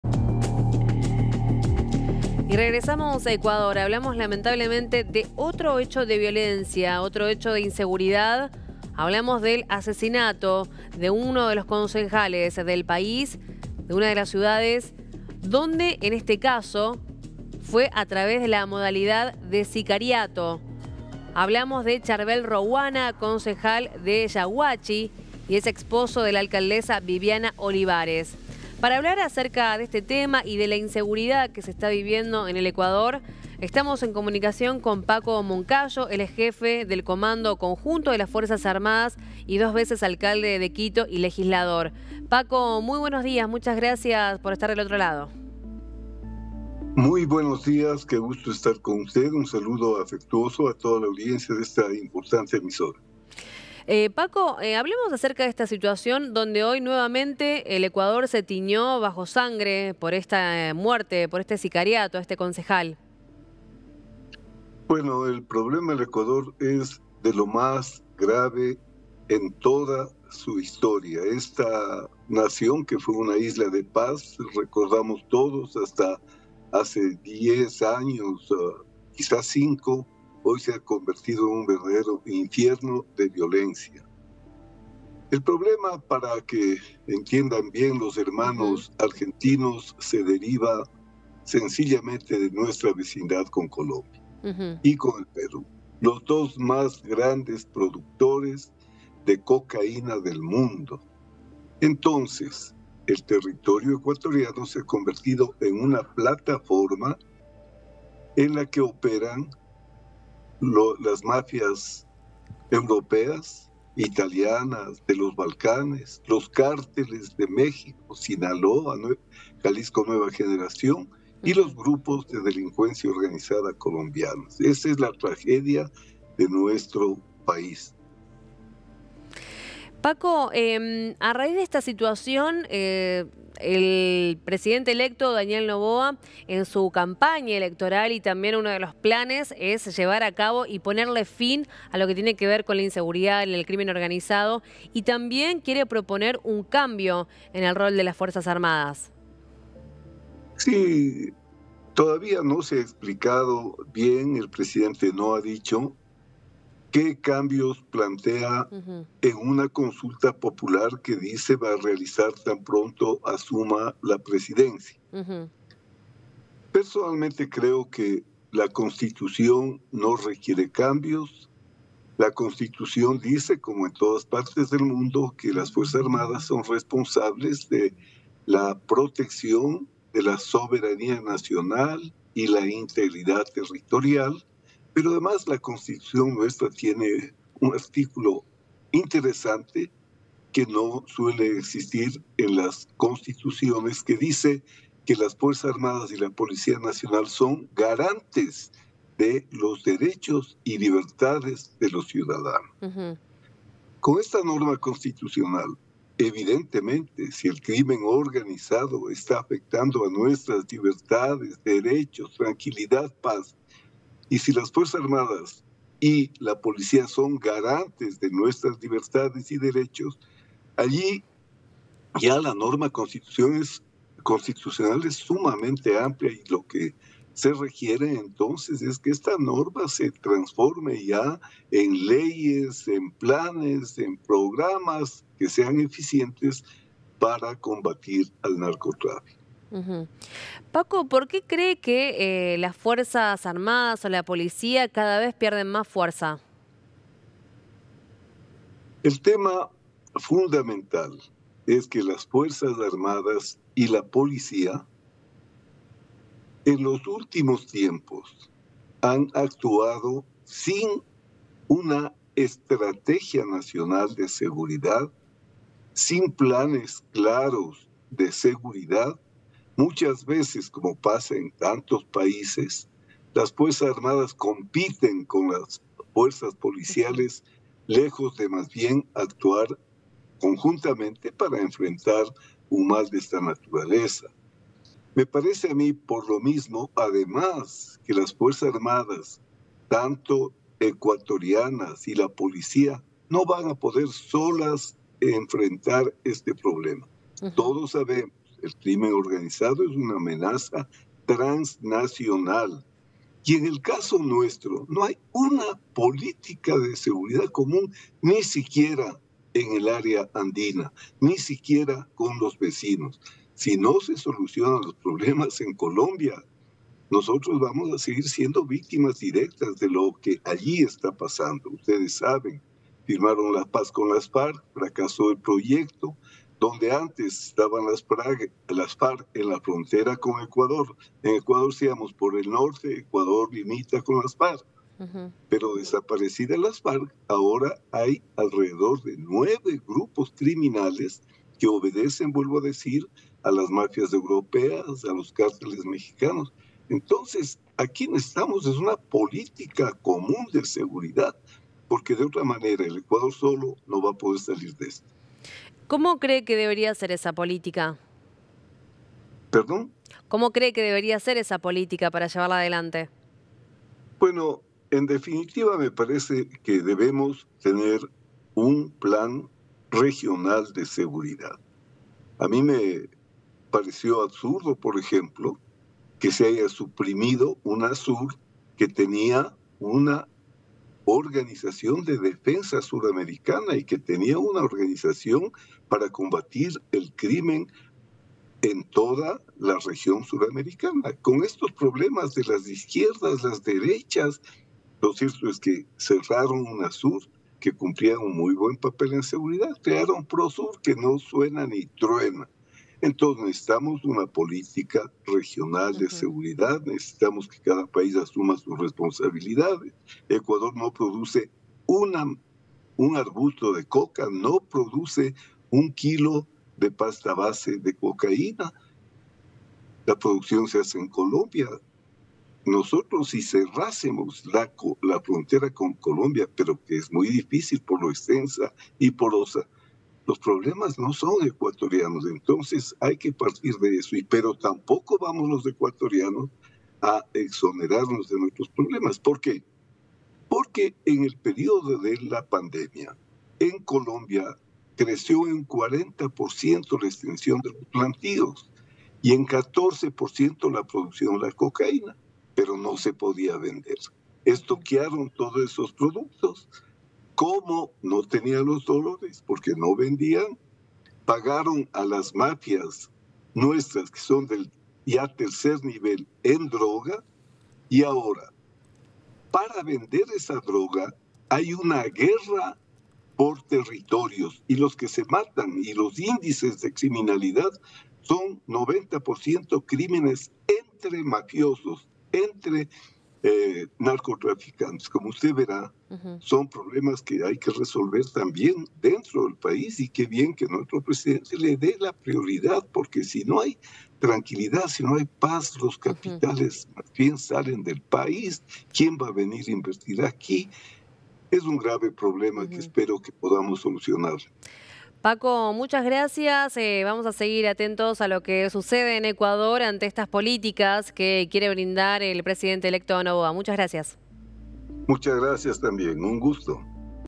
Ecuador: el concejal Charbel Rohuana fue asesinado bajo modalidad de sicariato
Magalí Schwartzman dialogó con el exjefe del comando conjunto de las Fuerzas Armadas de Ecuador, Paco Moncayo, para profundizar acerca del asesinato del concejal Charbel Rohuana, dentro del contexto de violencia política que vive Ecuador en los últimos tiempos.